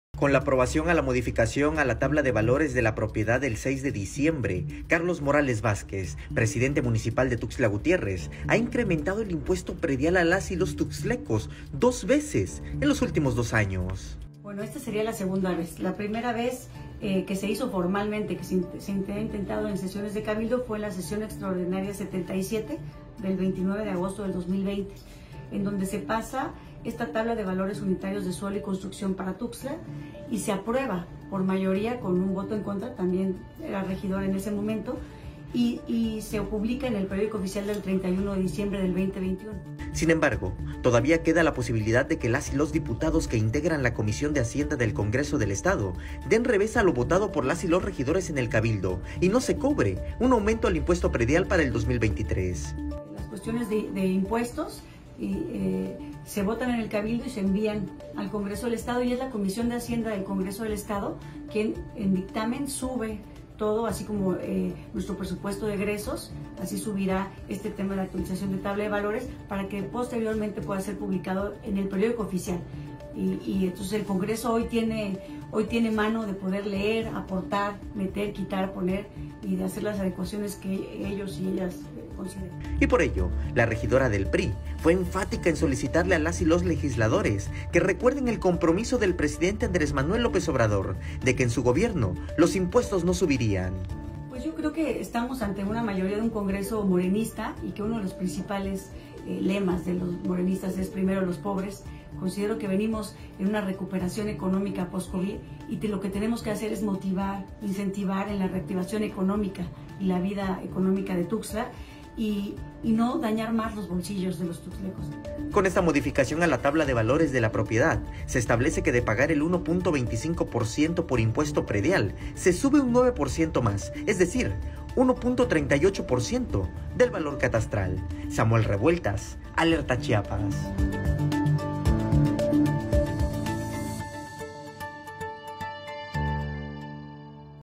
Carlos Morales ha incrementado el impuesto predial en los últimos dos años
Con la aprobación a la modificación a la tabla de valores de la propiedad el 6 de diciembre, el presidente del H. Ayuntamiento de Tuxtla Gutierrez., ha incrementado el impuesto predial dos veces en los últimos dos años.